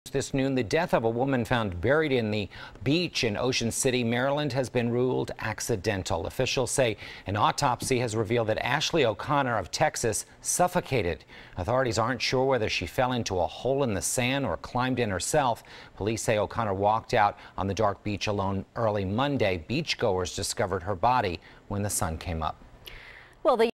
Woman's Death On Maryland Beach Ruled Accidental
CBS3's Jim Donovan reports.